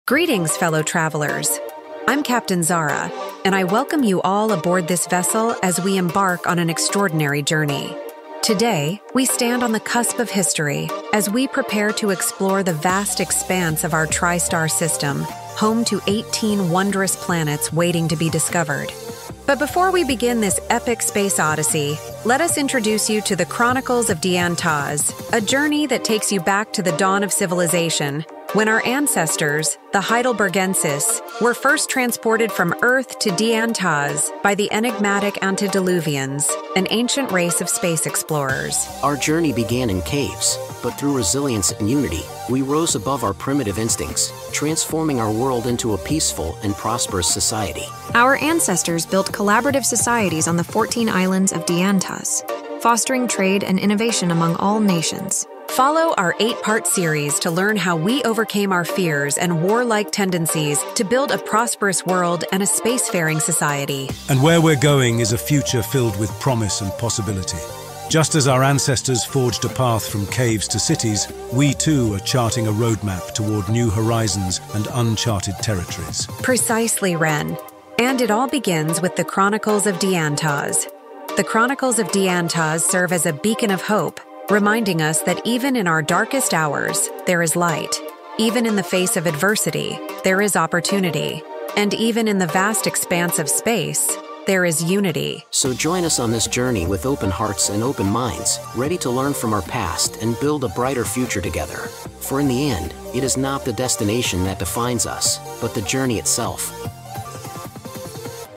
Chronicles of DianToz-Intro
Meet the crew of the starship Eridanus and take part in A Space Odyssey for the ages that all began with a visit from an alien race to the planet earth 200,000 years ago.